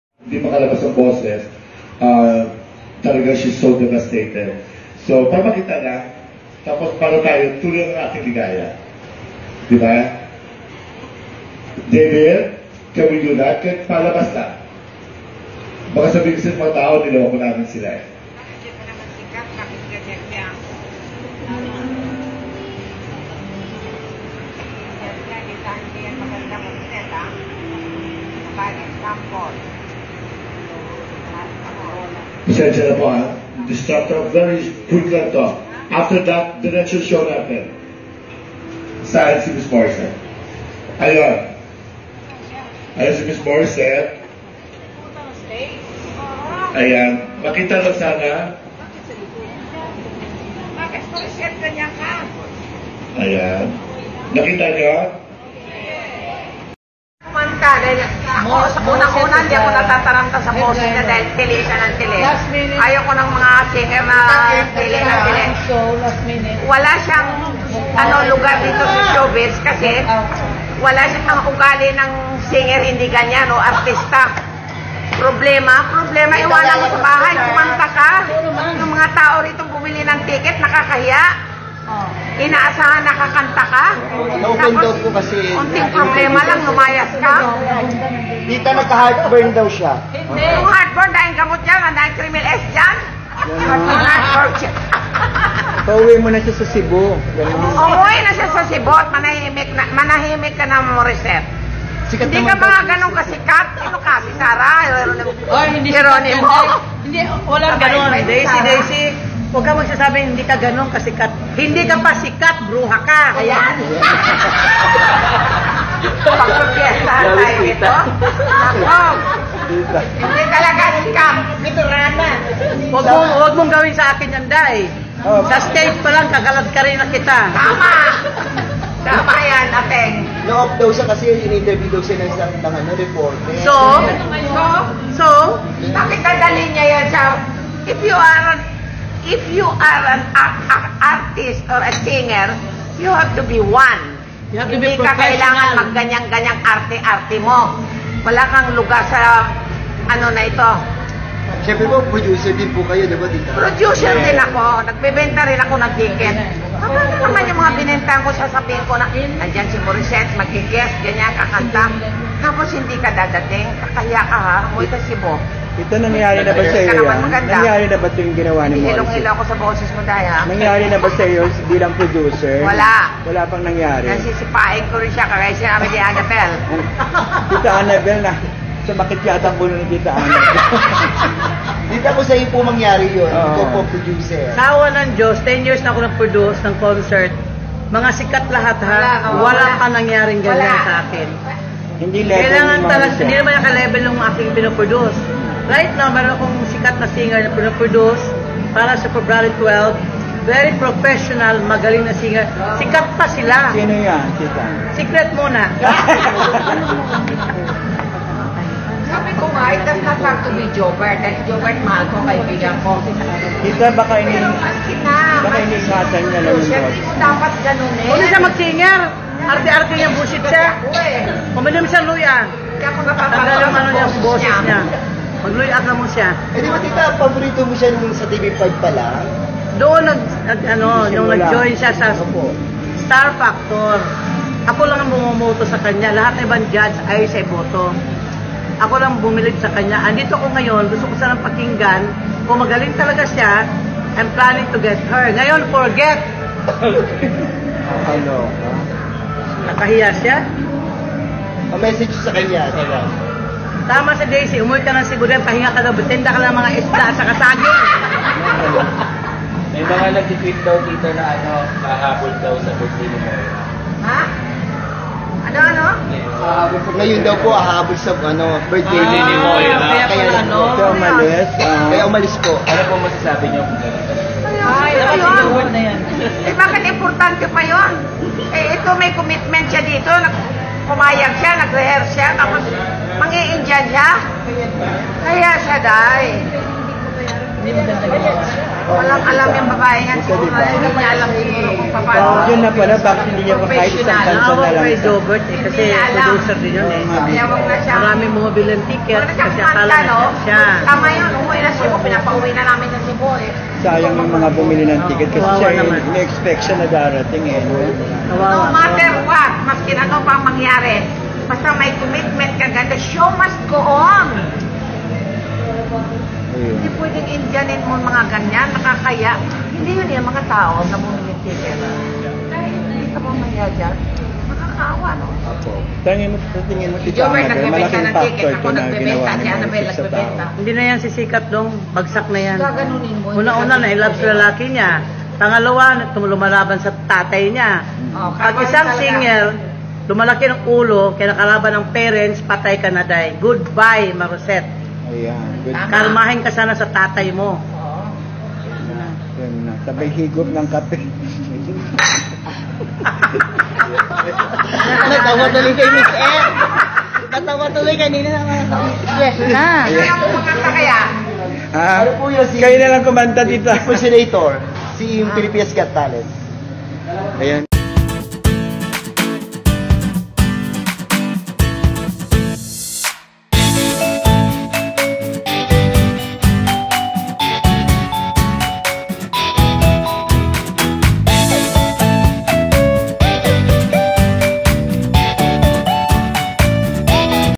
Ang pag walk-out ni Morissette | Annabelle Rama at Daisy Romualdez Galit na Galit at may bwelta
Ang pag walk-out ni Morissette kuhang-kuha ng isang audience sa loob Stadium | Annabelle Rama at Daisy Romualdez Galit na Galit at may bwelta sa singer.